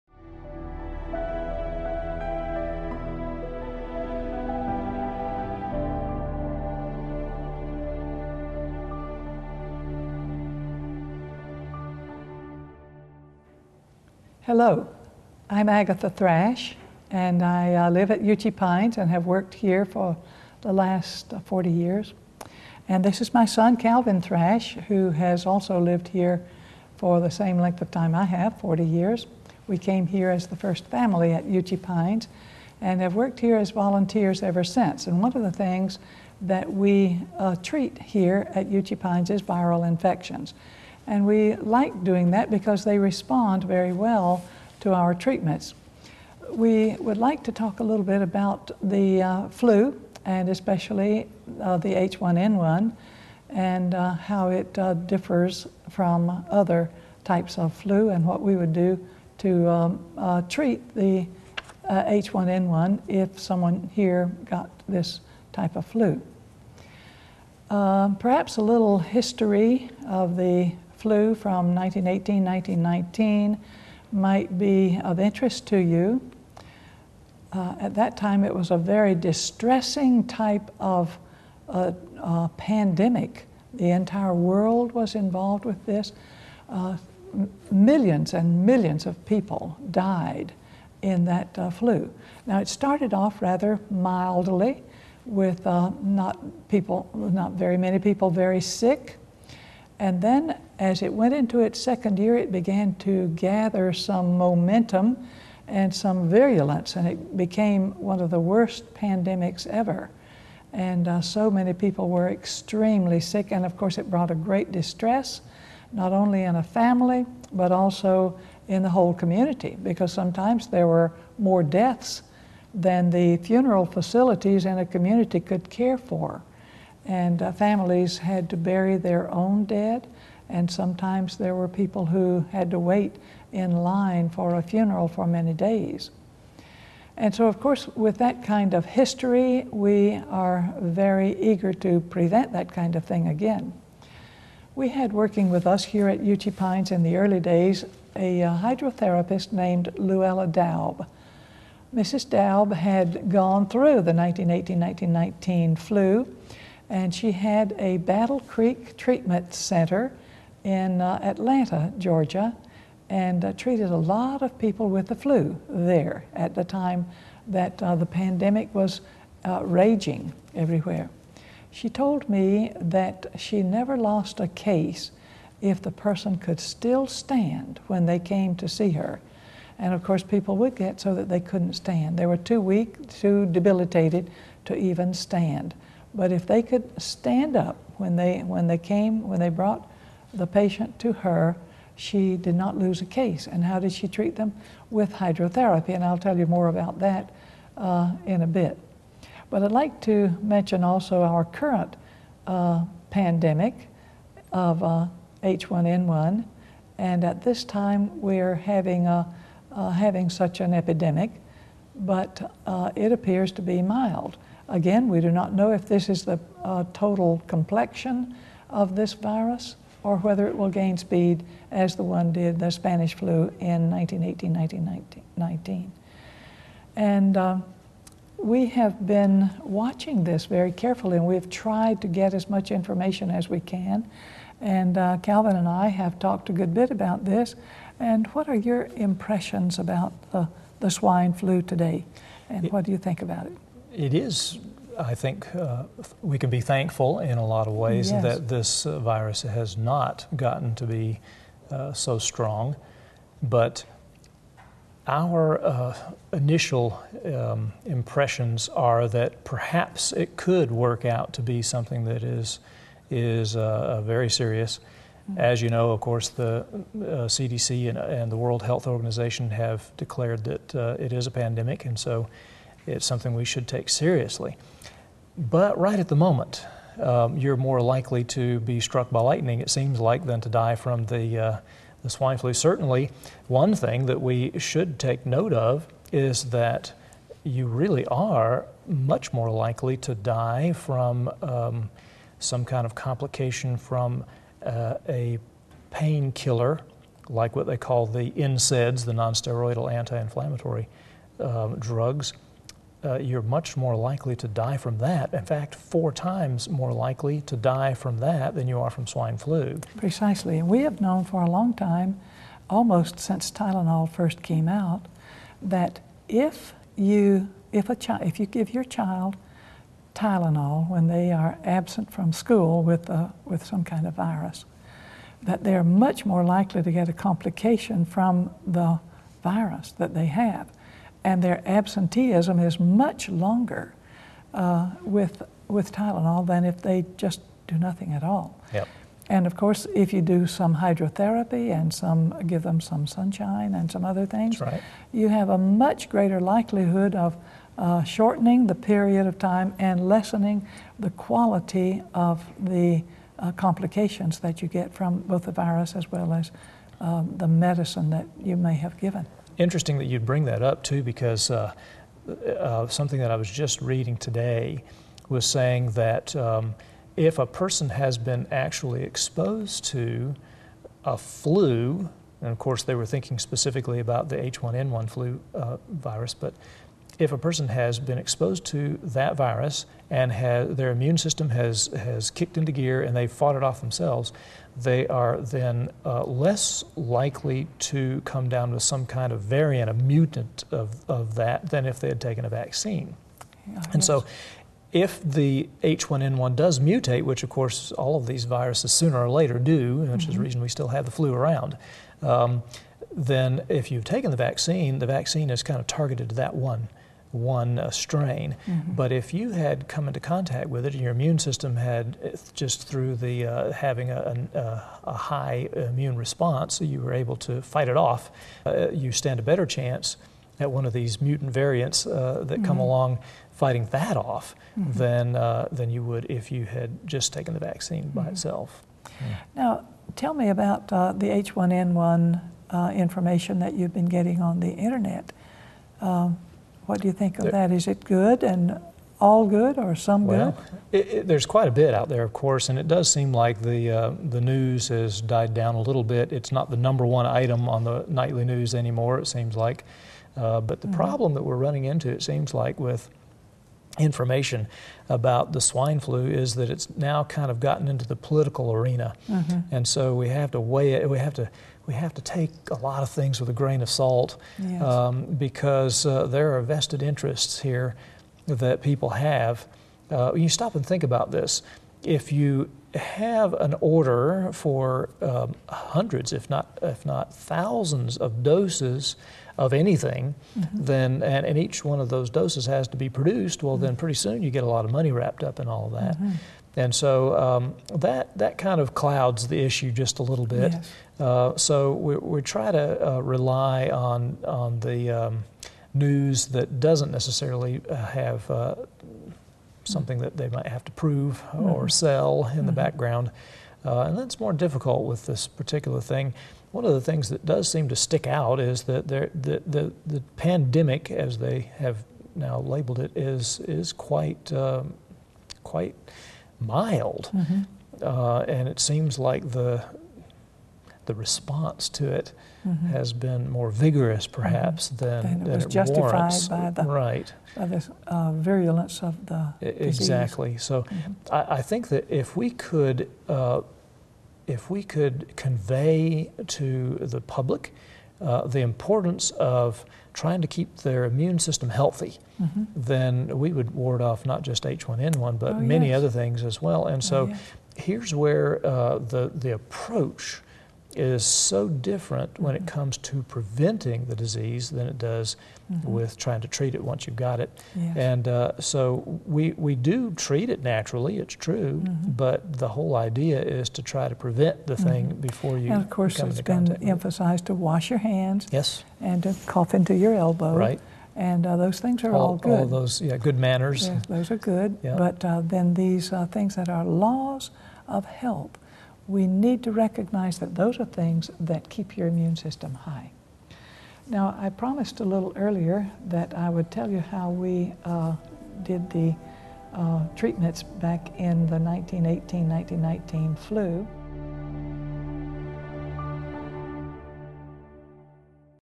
H1N1 Natural Remedies - Part 1
Agatha Thrash, MD from Uchee Pines Inst. is your host in this series of programs on dealing with H1N1 and other pandemics naturally. Learn fascinating info about how people have dealt with pandemics successfully in the past using such simple methods as hydrotherapy (also known as water therapy). And find the info you need to be aware of regarding the H1N1 vaccine as well as vaccines in general. In part 1, we learn some important background information about the "swine flu" or H1N1 before we begin treatments.